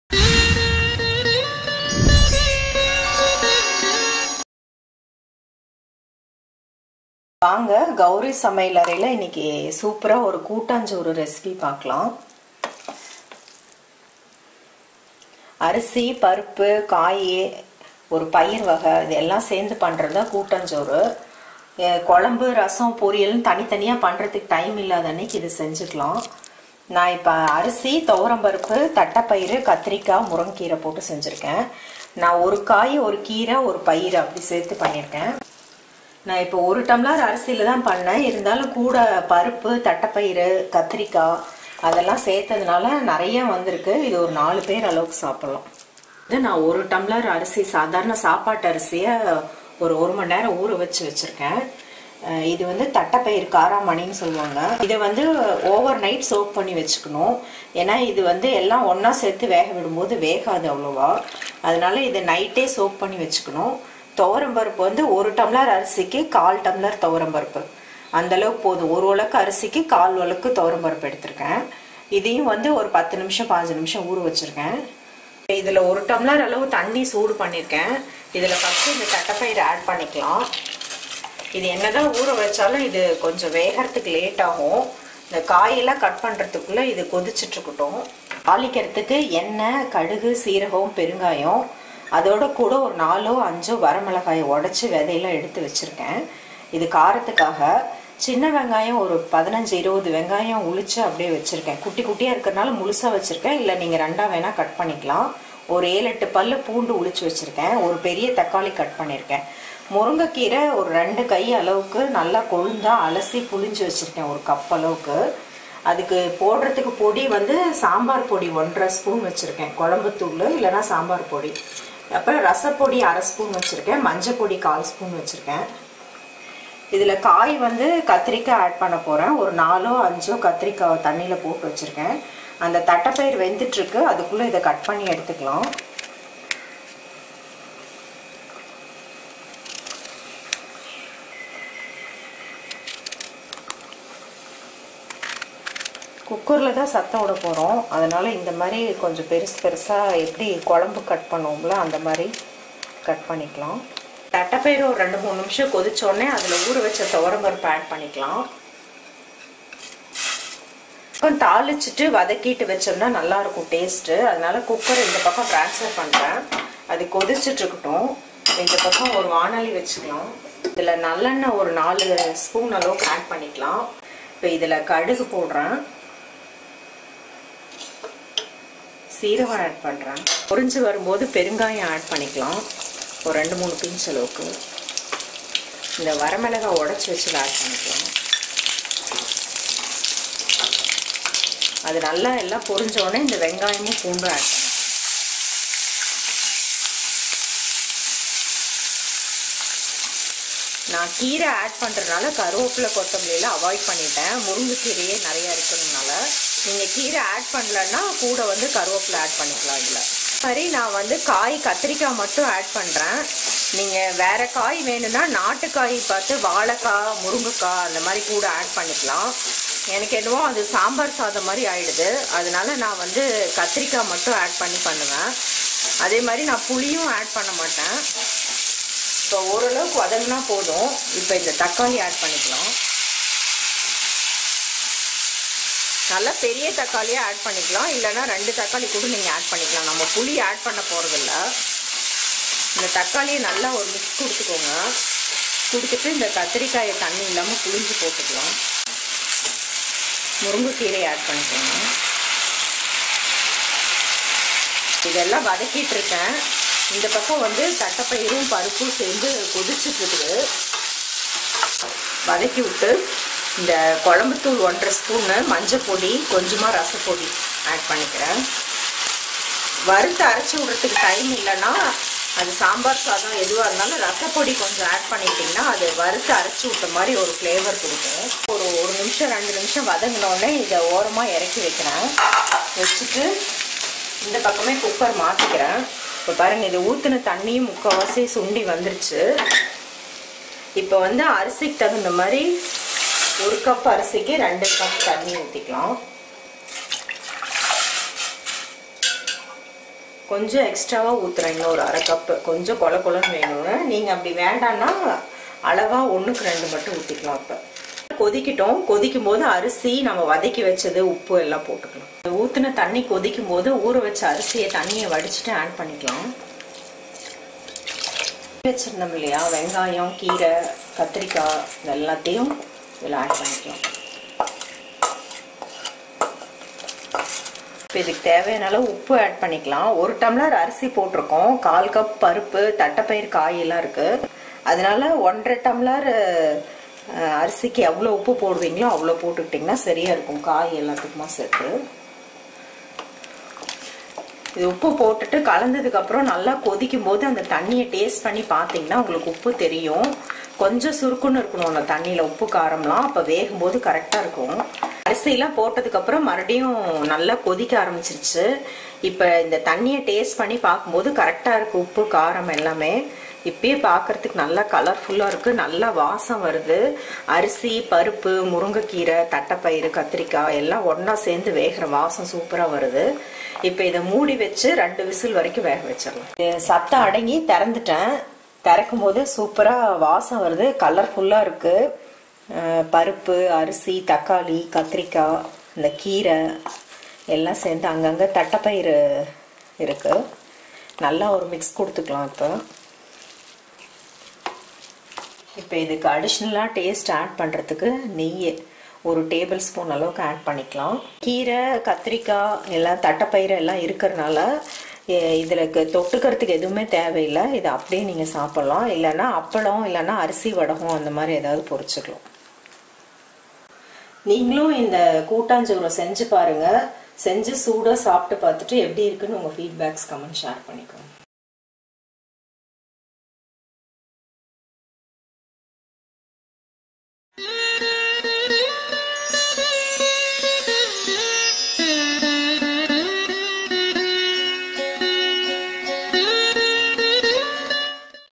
Kottan choru In Tamil | Mixed Rice Recipe | Healthy Complete Meals | Gowri Samayalarai
Mixed Rice is a quick, complete recipe.. Which contains rice, dhal,cow peas vegetable, greens..

Ingredients

Rice - 1 cup  (200 gm)

Thour dhal  - 1/4 cup

Karamani  (cow peas) - 1/4 cup

Brinjal  -  5 (200gm)

Drumstick leaves  - 1 cup

Sambar powder - 1and 1/2 tsp

Rasam powder -  1/2 tsp

Salt,  oil,  mustard seeds, jeera asfotida,  ghee

Steps

Soak karamani  for 8 hours. Soak rice for 1 hour.  Soak Thour dhal for  15 minutes.

Boil 1 glass of water in a cooker, add Karamani, And boil it for 3 minutes. Add Thour dhal and again boil it for  5 minutes.

In  4 spoon of oil fry little mustard seeds, jeera, asfotida, red chilly. Add onions, garlic and fry for a minute, add tomato and mix well,  add Brinjal,  drumstick leaves, all powders and mix well. Keep aside.

Now in cooker add 2  glasses of water, when it boils add rice,  all sauted things and salt. Cook for 2 whistles.

Open add a tbs of ghee and mix well., ,,

To buy products hand picked by me in Amazon

Refer Gowri Samayalarai For

Presto
Kitchen Towel 80 Pulls × 2 Rolls

Presto
Kitchen Towel 80 Pulls one Roll